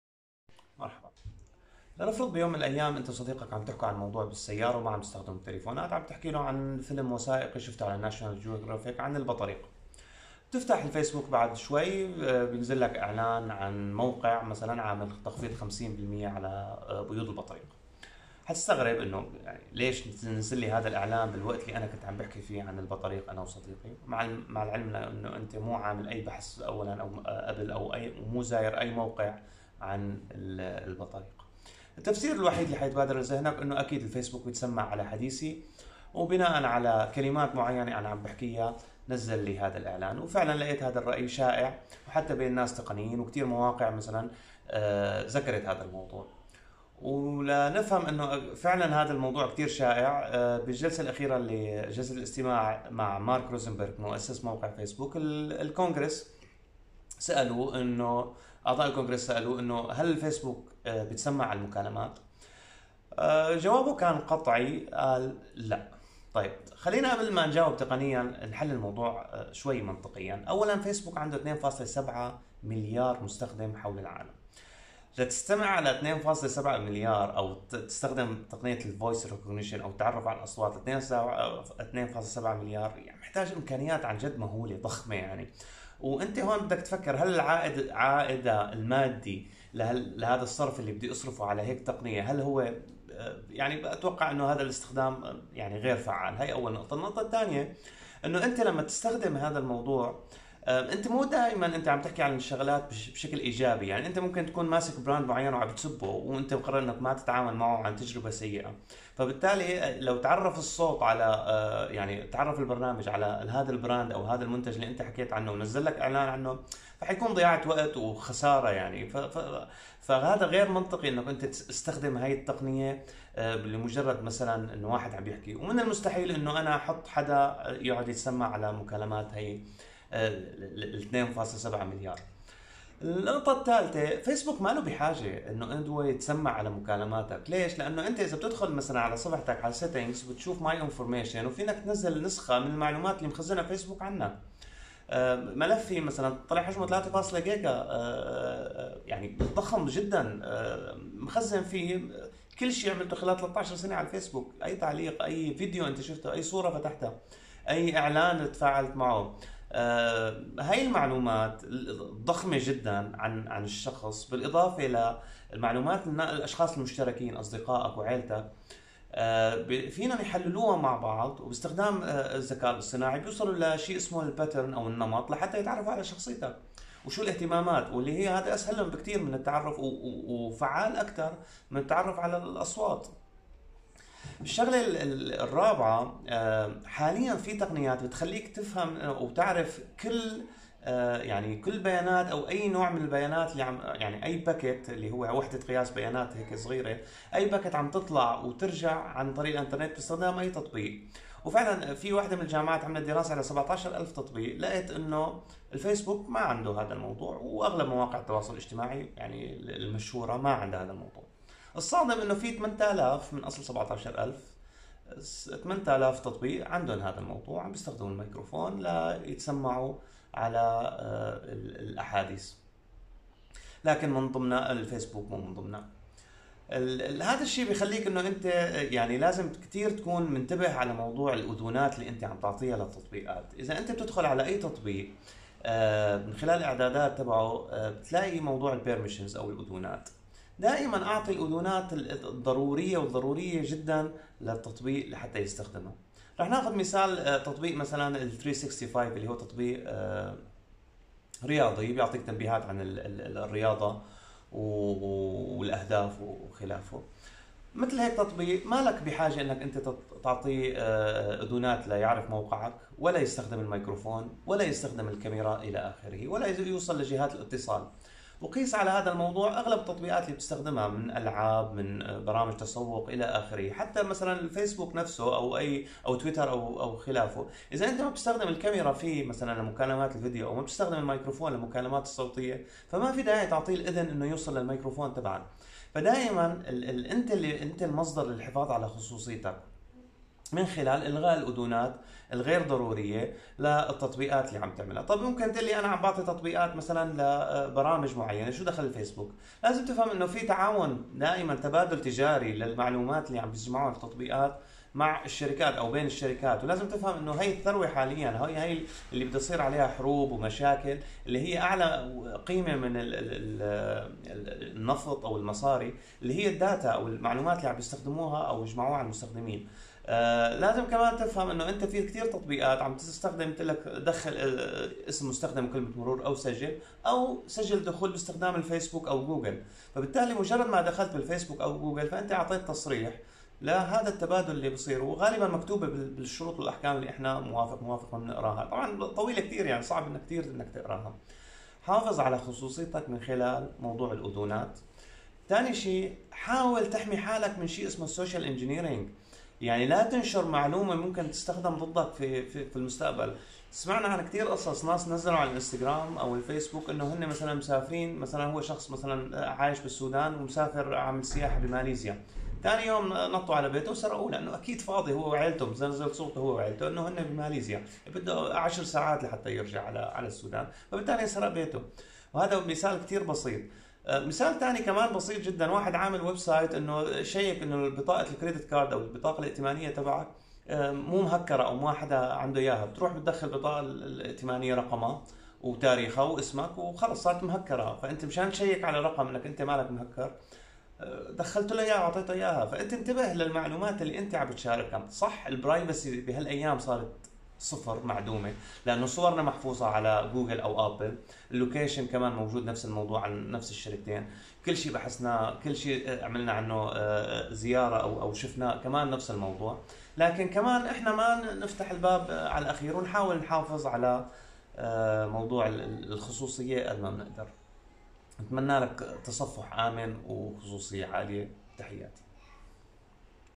هل الفيس بوك يسجل المكالمات و يستمع لمحادثاتنا؟
هل يستمع الفيس بوك لمحادثاتنا؟ هل الفيس بوك يسجل المكالمات